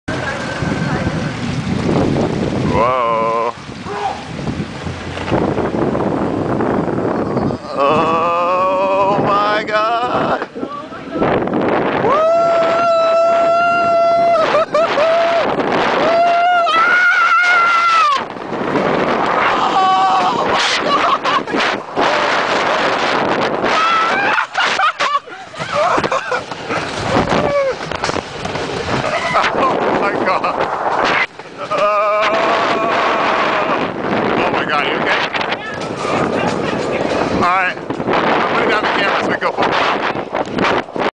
Jamaican Bobsled Ride Action Video
The ride was way more insane than I expected it to be, and I was the one driving. We obviously went full-throttle the entire time and I am completely shocked that we stayed on the track. I had to turn the camera off halfway through. We ended up going back for a second ride, once the color returned to Meg's face.